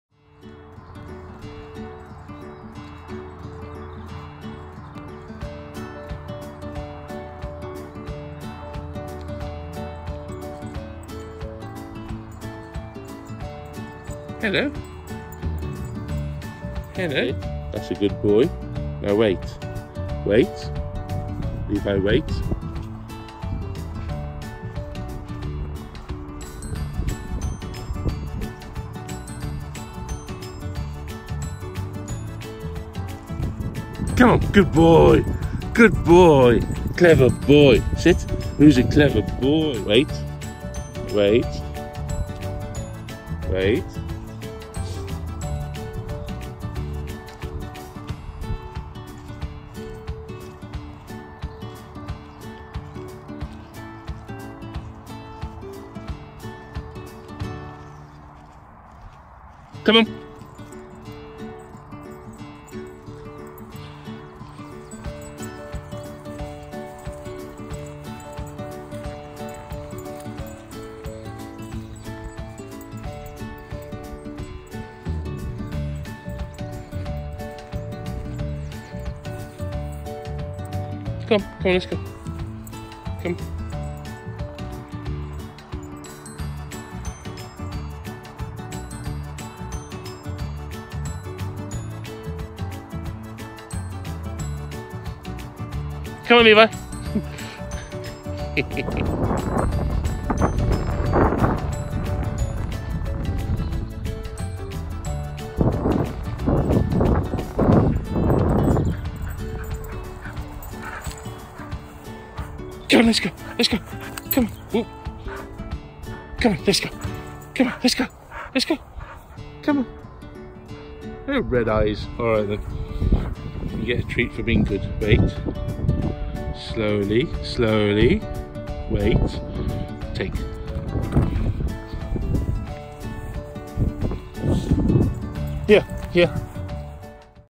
Old Dog Turning Tricks 😂
Levi running around off lead and obedience training